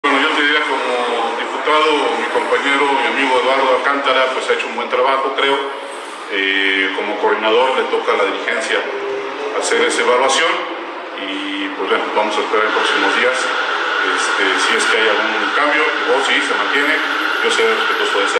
La dirigencia del PAN debe evaluar el desempeño de Eduardo Alcántara como líder de bancada: Micalco
La dirigencia del PAN debe evaluar el desempeño de Eduardo Alcántara como líder de la bancada del PAN: Rafael Micalco